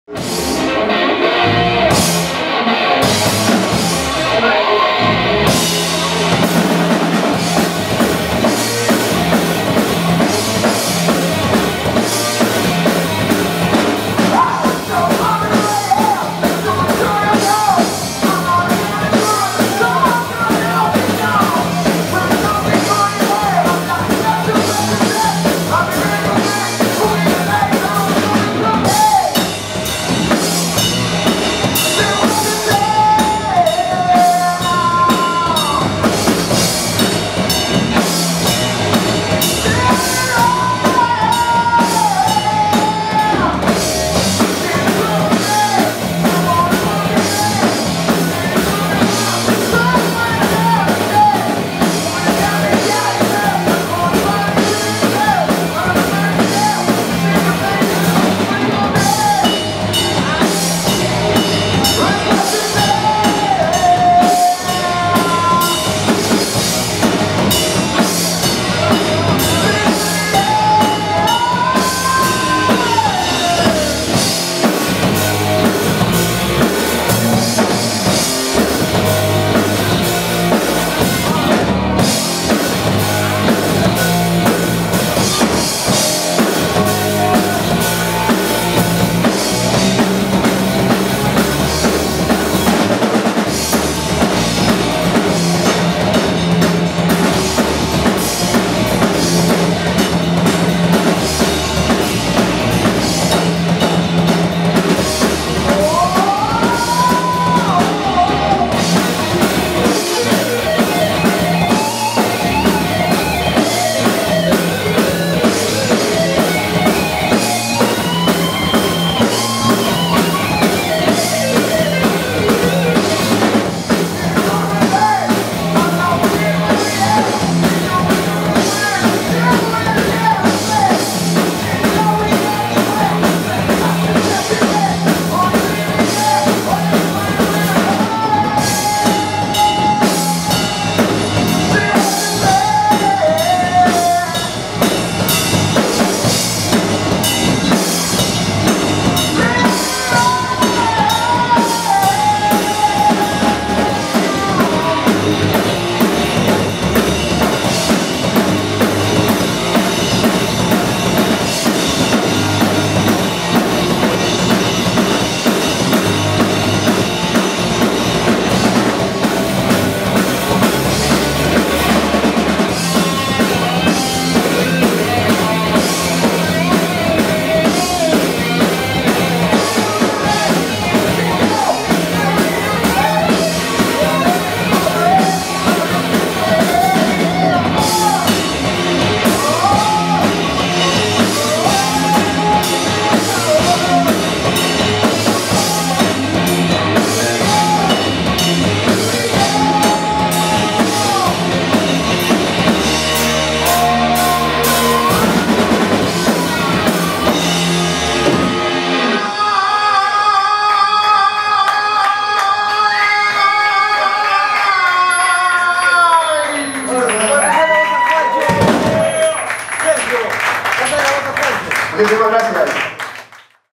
VICIOUS BOOTLEG INEDITO
VIDEO INEDITO DE LA BANDA VICIOUS EN EL YA DESAPARECIDO KEKO BAR DE BARRANCO.